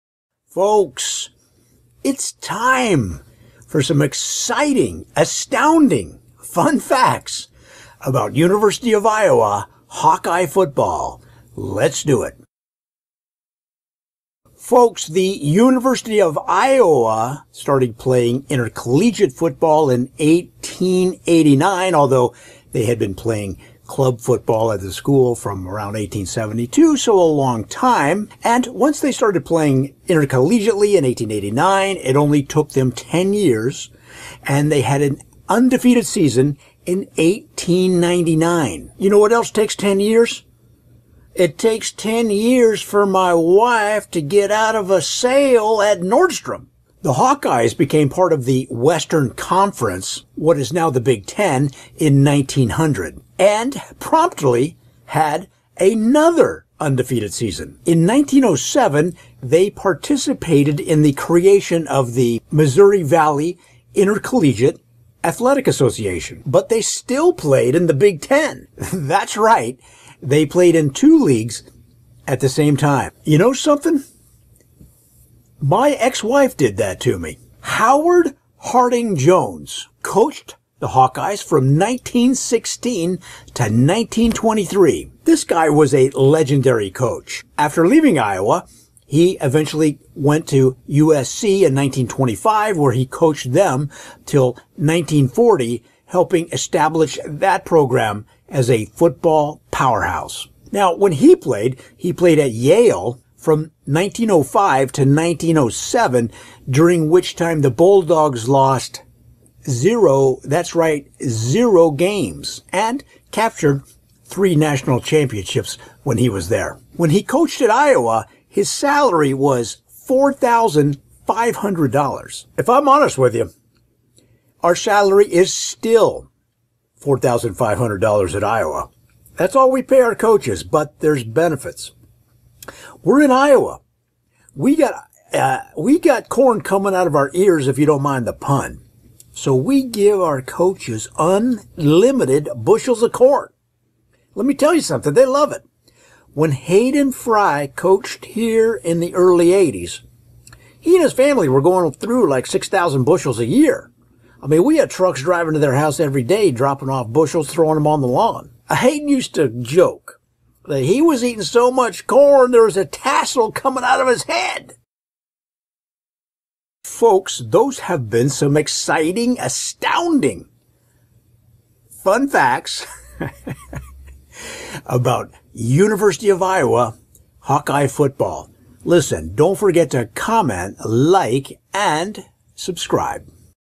Astounding Fun Facts About University of Iowa Football | JOE DITZEL HAS SOME PROBLEMS
Joe Ditzel is a humorist, author and keynote speaker.

Astounding Fun Facts About University of Iowa Football

Iowa started playing intercollegiate football in 1889, although club teams had been playing at the school since 1872.  It only took them 10 years to record an undefeated season.

"You know what else takes 10 years? Waiting for my girlfriend outside Nordstrom during a sale."

The Hawkeyes became part of the Western Conference (what is now the Big 10) in 1900, and promptly had another undefeated season. In 1907 they participated in helping create another league called the Missouri Valley Intercollegiate Athletic Association, but they still stayed part of the Big 10. That's right, they played in two conferences at the same time.

"My ex-wife did that to me."

Howard Harding Jones coached the Hawkeyes from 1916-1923. He was a legendary coach, going to coach USC for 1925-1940, helping them become a football powerhouse. He played at Yale from 1905 to 1907, during which the Yale Bulldogs lost 0, that's right 0 games and grabbed national championships every year. His salary at Iowa was $4,500.

"Actally, we still only pay our coaches $4,500. But this is Iowa, we let you have all the corn you want for free. When Hayden Fry was coach in the early 80s, his family was going through maybe 6000 bushels of corn a year."